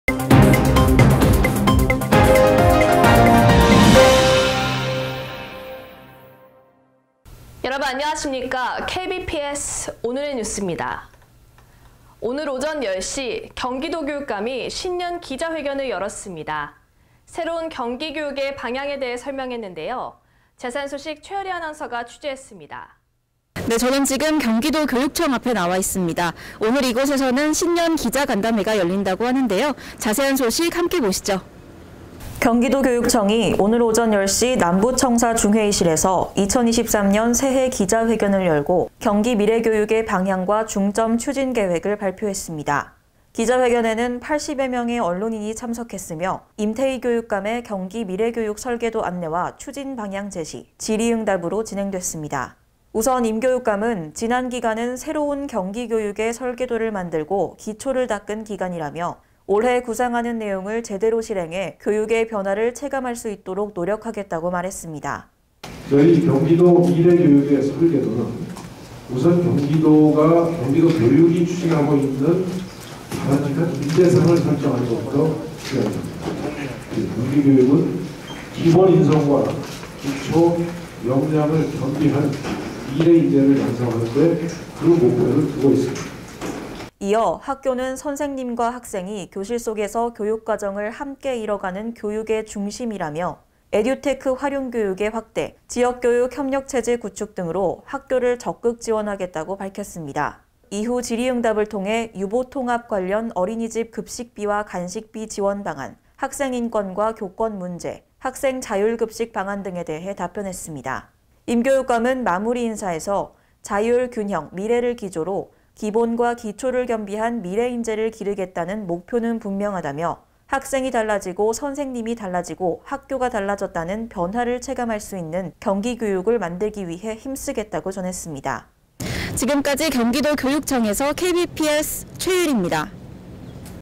경기도 임태희 교육감, 경기 미래 교육 방향 제시
경기도교육청(교육감 임태희)이 오늘 오전 10시 남부청사 중회의실에서 ‘2023년 새해 기자회견’을 열고 경기미래교육의 방향과 중점 추진 계획을 발표했다.

기자회견에는 80여 명의 언론인이 참석했으며, 임태희 교육감의 ‘경기 미래 교육 설계도’ 안내와 추진 방향 제시, 질의응답으로 진행됐다.

우선 임 교육감은 “지난 기간은 새로운 경기교육의 설계도를 만들고 기초를 닦은 기간”이라며 “올해 구상하는 내용을 제대로 실행해 교육의 변화를 체감할 수 있도록 노력하겠다”고 말했다.

이어 “학교는 선생님과 학생이 교실 속에서 교육과정을 함께 이뤄가는 교육의 중심”이라며 에듀테크 활용교육의 확대, 지역교육협력 체제 구축 등으로 학교를 적극 지원하겠다고 밝혔다.

이후 질의응답을 통해 유보통합 관련 어린이집 급식비와 간식비 지원방안, 학생 인권과 교권 문제, 학생 자율급식 방안 등에 대해 답변했다.

임 교육감은 마무리 인사에서 “자율, 균형, 미래를 기조로 기본과 기초를 겸비한 미래인재를 기르겠다는 목표는 분명하다”며 “학생이 달라지고, 선생님이 달라지고, 학교가 달라졌다는 변화를 체감할 수 있는 경기교육을 만들기 위해 힘쓰겠다”고 전했다.